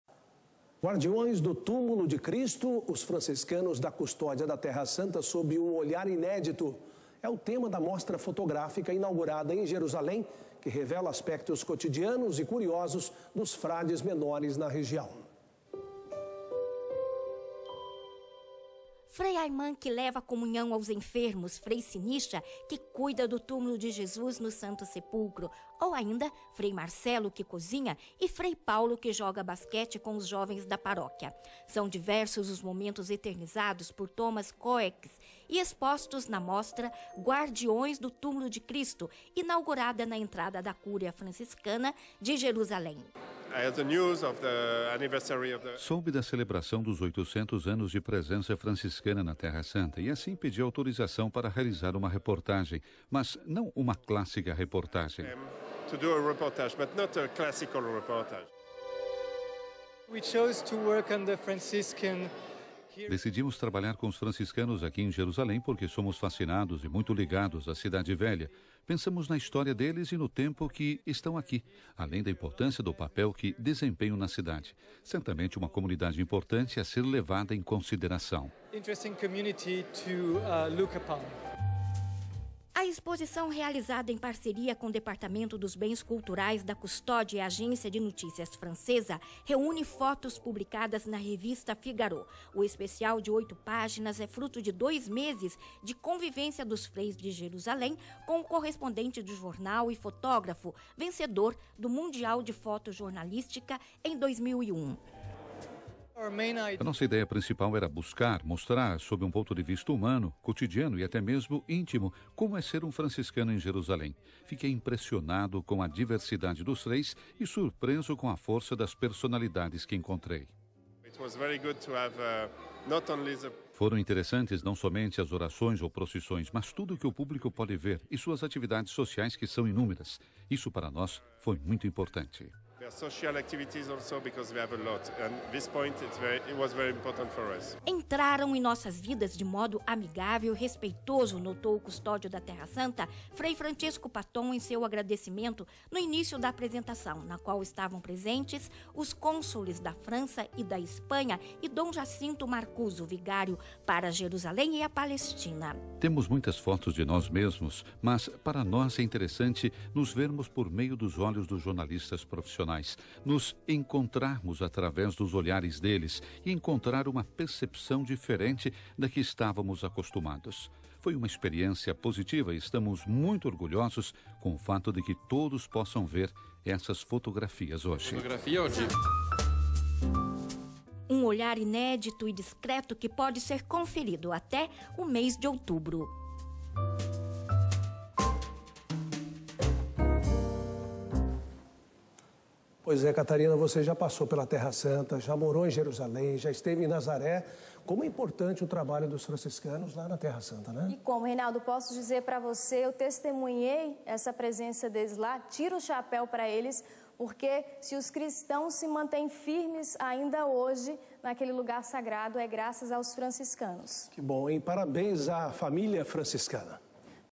Mostra fotográfica revela o trabalho dos franciscanos na Terra Santa
"Guardiões do túmulo de Cristo", os franciscanos da Custódia da Terra Santa sob um olhar inédito. É o tema da mostra fotográfica inaugurada em Jerusalém que revela aspectos cotidianos e curiosos dos frades menores na região.

Reportagem:  Ilaria Cesanelli
Narração:  Lurdinha Nunes
Imagens:  Joseph Safar
Edição:  Miriam Bianchi